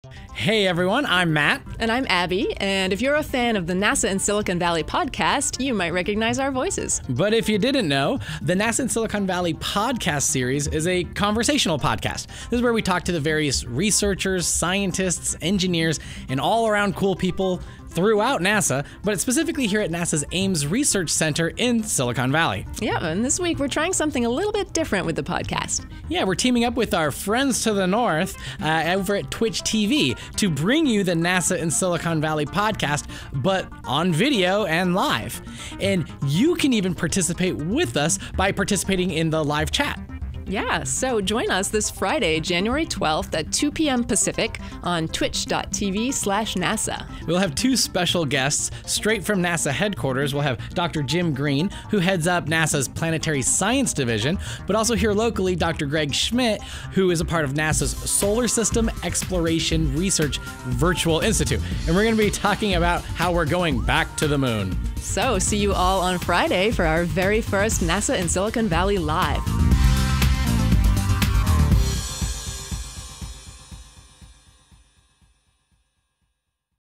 Coming Soon to Twitch: NASA in Silicon Valley Live
We’re launching a new live show on Twitch called NASA in Silicon Valley Live! The show is a spin-off of our weekly audio show called the NASA in Silicon Valley podcast, which features conversations with the various scientists, engineers, researchers and all around cool people at NASA. This week, we’re talking about going back to the Moon with NASA rock stars Jim Green and Greg Schmidt. Tune in Friday, January 12 at 2 pm PT / 5 pm ET and join the conversation

Video credit: NASA/Ames Research Center

NASA's Ames Research Center is located in California's Silicon Valley.